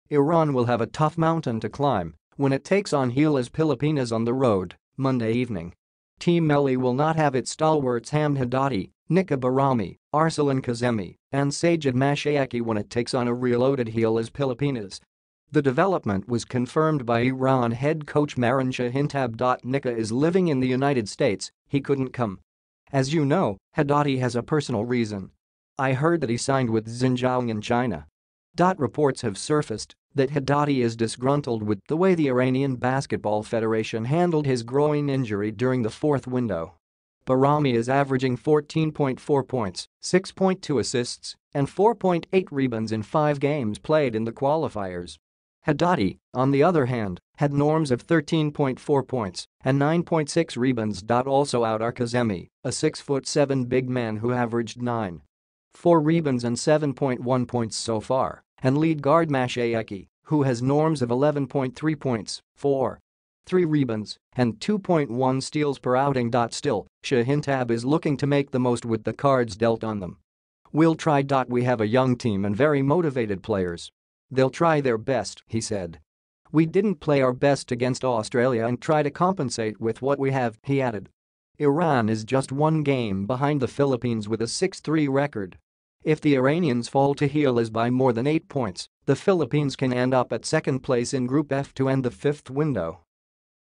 Hamed Haddadi, Nikkah Bahrami out of Iran lineup
Iran will have a tough mountain to climb when it takes on Gilas Pilipinas on the road, Monday evening.

Team Melli will not have its stalwarts Hammed Haddadi, Nikkah Bahrami, Arsalan Kazemi, and Saijid Mashayekhi when it takes on a reloaded Gilas Pilipinas.

The development was confirmed by Iran head coach Mehran Shahintab.

“Nikkah is living in the United States, he couldn’t come. As you know, Haddadi has a personal reason. I heard that he signed with Xinjiang in China.”

Reports have surfaced that Haddadi is disgruntled with the way the Iranian basketball federation handled his groin injury during the fourth window.

Bahrami is averaging 14.4 points, 6.2 assists, and 4.8 rebounds in five games played in the Qualifiers. Haddadi, on the other hand, had norms of 13.4 points and 9.6 rebounds.

Also out are Kazemi, a 6-foot-7 big man who averaged 9.4 rebounds and 7.1 points so far, and lead guard Mashayekhi, who has norms of 11.3 points, 4.3 rebounds, and 2.1 steals per outing.

Still, Shahintab is looking to make the most with the cards dealt on them.

“We’ll try.

“We have a young team and very motivated players. They’ll try their best,” he said.

“We didn’t play our best against Australia and try to compensate with what we have,” he added.

Iran is just one game behind the Philippines with a 6-3 record. If the Iranians fall to Gilas by more than eight points, the Philippines can end up at second place in Group F to end the fifth window.